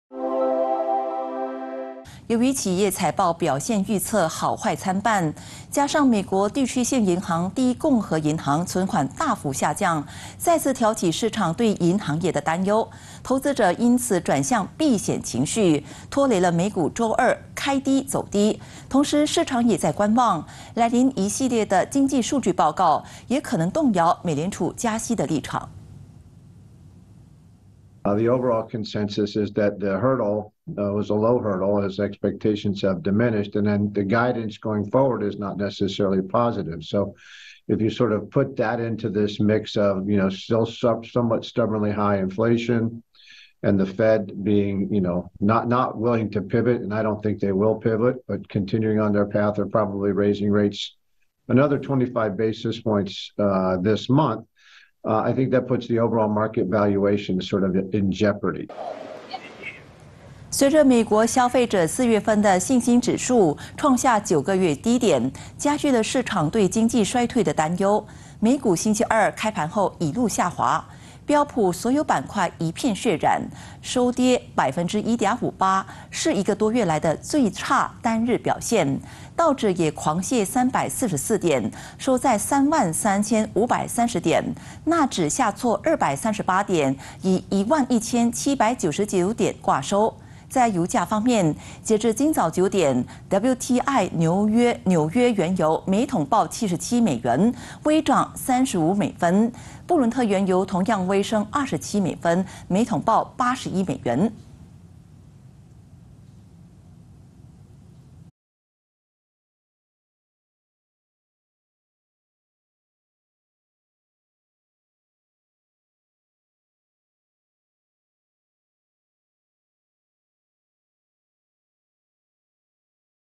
《股汇动态》对前景忧虑转向避险情绪 三大美指周二开低走低【2023.04.26 八度早新闻】
【对前景忧虑转向避险情绪  三大美指周二开低走低】
由于企业财报表现预测好坏参半，加上美国地区性银行第一共和银行存款大幅下降，再次挑起市场对银行业的担忧，投资者因此转向避险情绪，拖累美股周二开低、走低。同时，市场也在观望来临一系列的经济数据报告，也可能动摇美联储加息的立场。

随着美国消费者四月份的信心指数、创下九个月低点，加剧了市场对经济衰退的担忧。美股星期二开盘后一路下滑，标普所有板块一片血染，收跌百分之1点58，是一个多月来的最差单日表现。道指也狂泻344点，收在3万3千530点；纳指下挫238点，以1万1千799点挂收。油价方面，截至今早九点，WTI纽约原油每桶报77美元，微涨35美分；布伦特原油同样微升27美分，每桶报81美元。